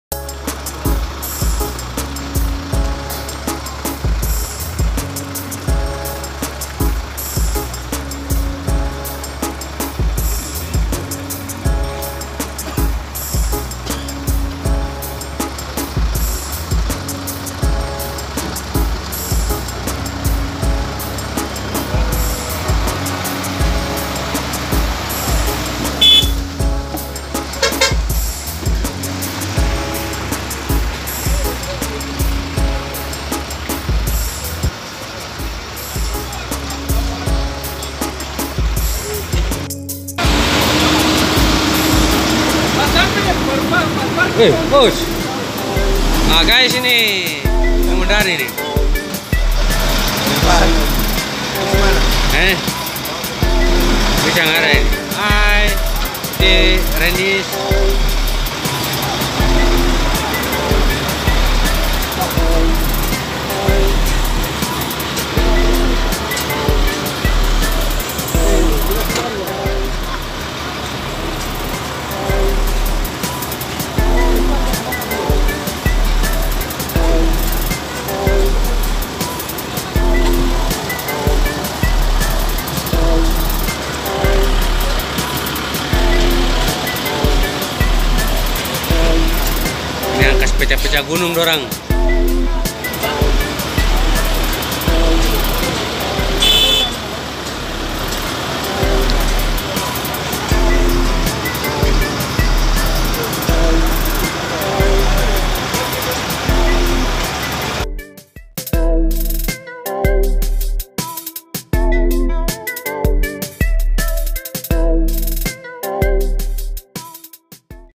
Freeport Trip Tembagapura Timika Papua
Perjalanan dari pusat pertambangan atau Tembagapura ke Timika Papua